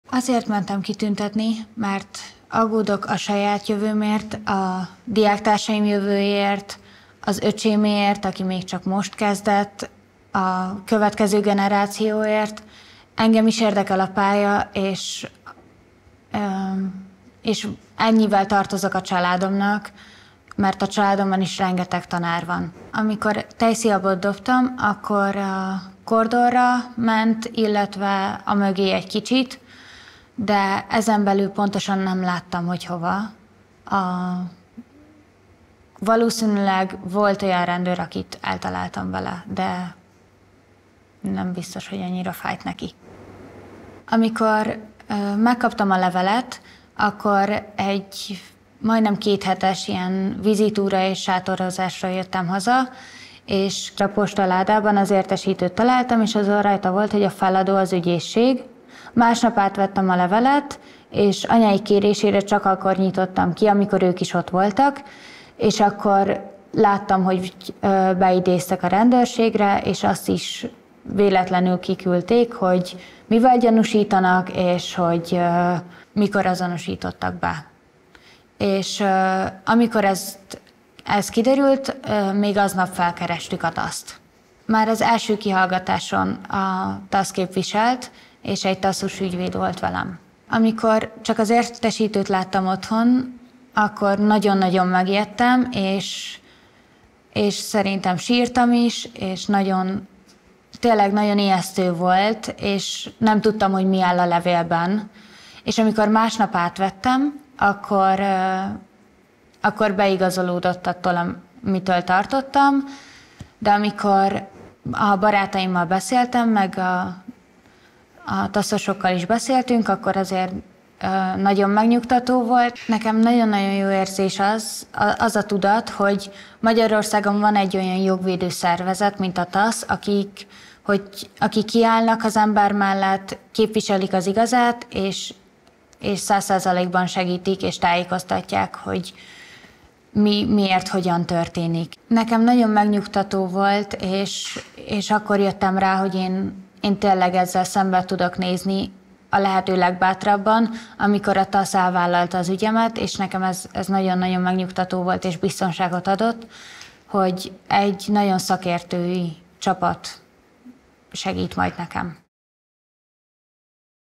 Az oktatásért tüntettem, most börtönnel fenyegetnek.
Sára gimnazistaként egy diáktüntetésen a rendőrök által bevetett könnygázra tejszínhabbal válaszolt. Most akár börtönbüntetést is kaphat. Nem hagyjuk, hogy Sárát, esetén keresztül pedig egy egész generációt megfélemlítsen és eltántorítson a hatalom attól, hogy hangot adjanak véleményüknek.

Téged is zavar? Támogasd a TASZ munkáját! tasz.hu/tamogatas

További infó:
tasz.hu/rolunk
ataszjelenti.444.hu/